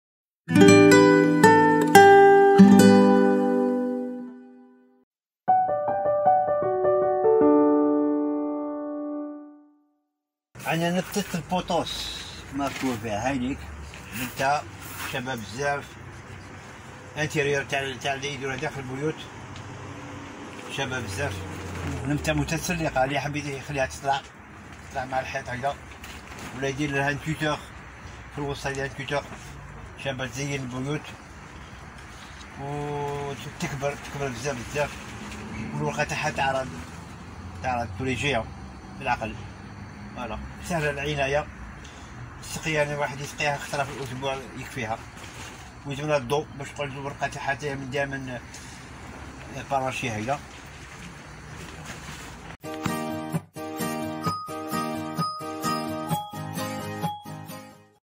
Tout Savoir sur le Pothos : La Plante d'Intérieur Facile à Entretenir
Découvrez le Secret du Pothos : La Plante Facile pour Décorer et Purifier Votre Espace 🌿✨
Dans cette vidéo, plongez dans l'univers de عمي محمود et découvrez cette belle plante du Pothos, une plante d'intérieur populaire et facile à entretenir ! Je vous explique comment prendre soin de cette plante merveilleuse, quels sont ses bienfaits pour purifier l'air et comment la faire prospérer chez vous. Que vous soyez débutant en jardinage ou un passionné de plantes, le Pothos est un excellent choix pour embellir votre intérieur !